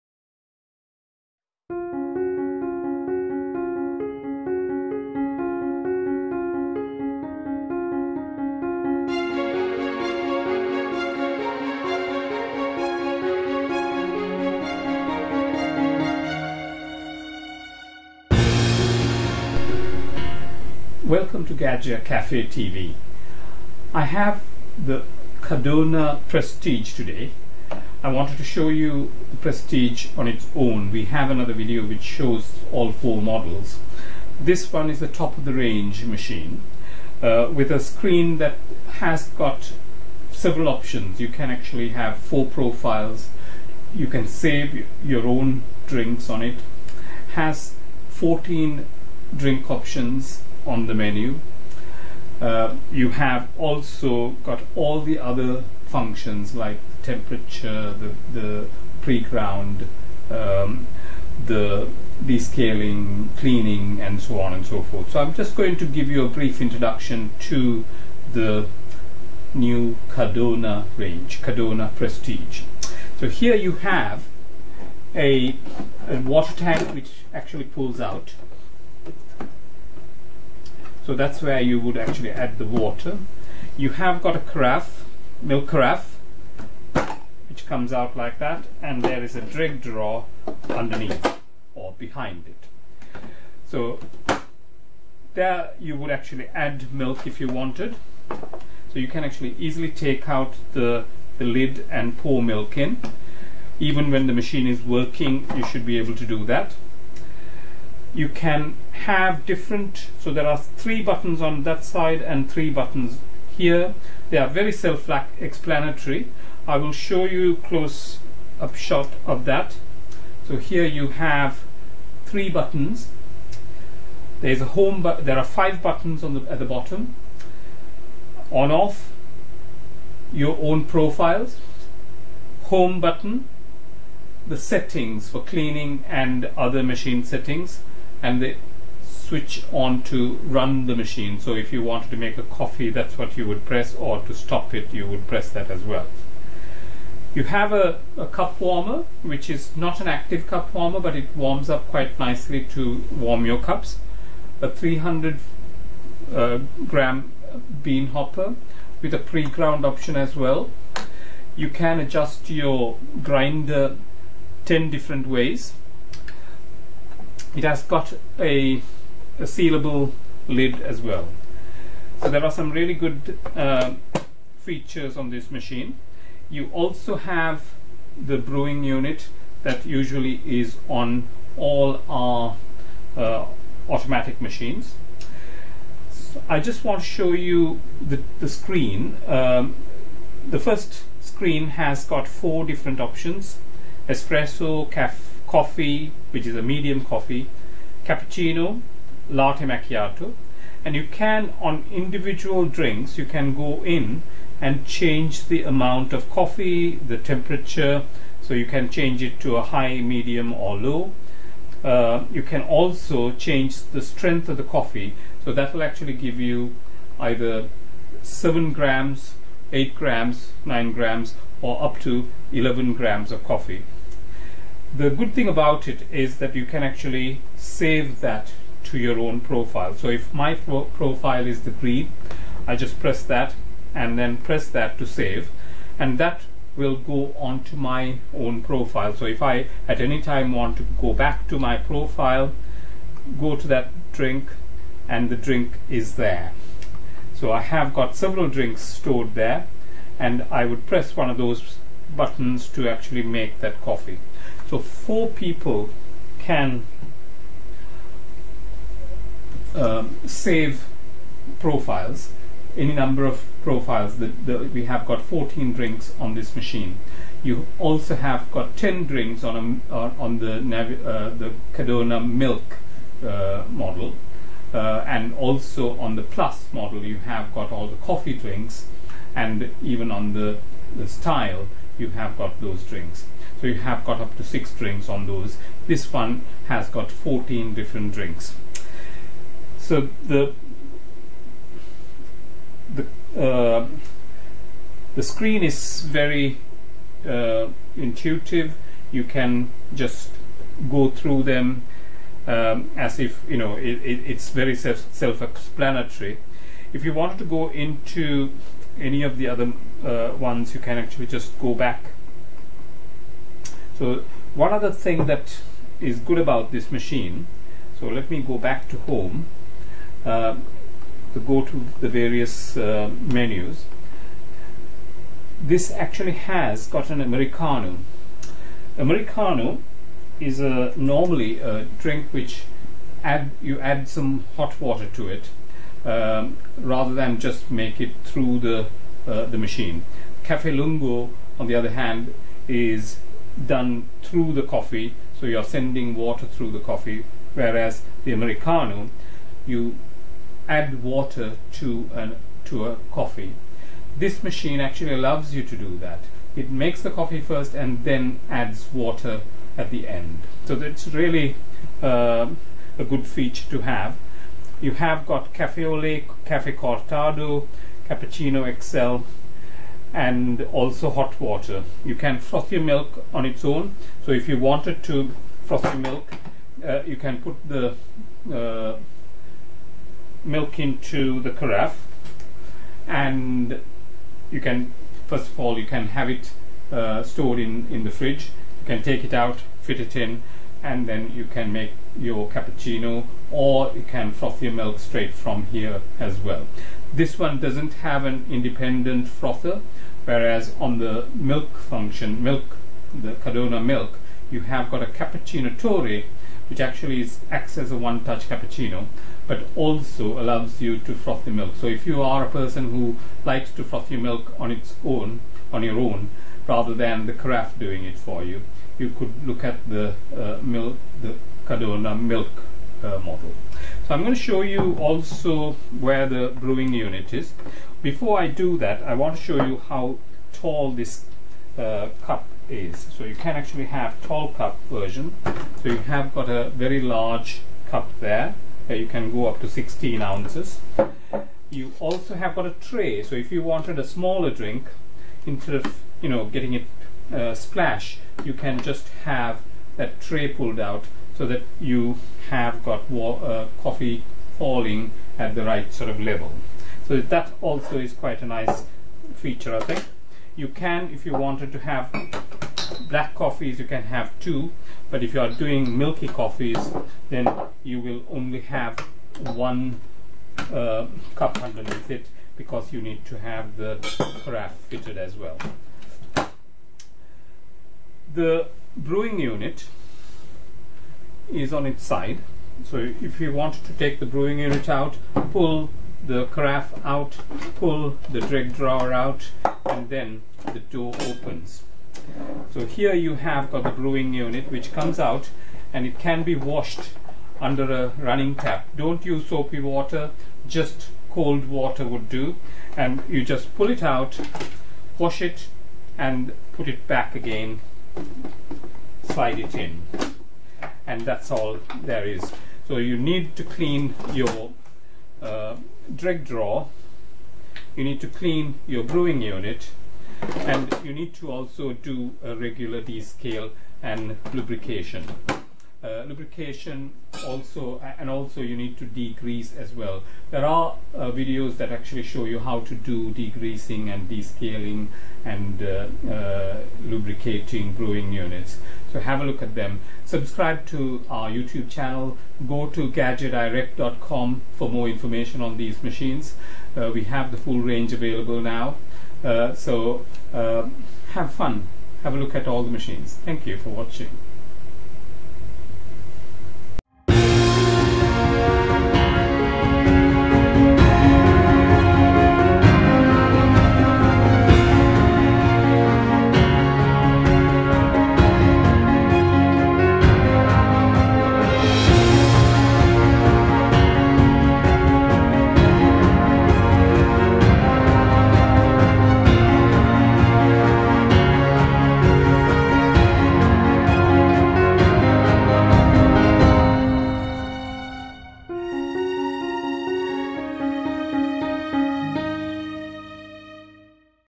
Gaggia Cadorna Prestige - Gaggia Caffe TV
Gaggia Cadorna Prestige bean to cup coffee machine. It takes you to a another level of coffee making.